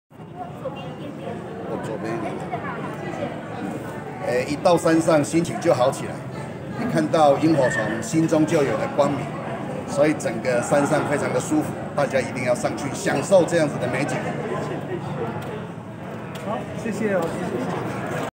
亞洲全球新聞（記者胡黎云，陳文娟/高雄市政府報導）
創意巧塔鬼滅原鄉螢光不滅2021那瑪夏賞螢季即將大發，那瑪夏季將於4月2日至5月1日浪漫登場，同時訂於4月17日專為你情侶或夫妻檔限量啓程（螢專車，為彼此許下祝福，留下永恆不減的美好回憶。
（新聞一刀未剪）你的支持是我們的動力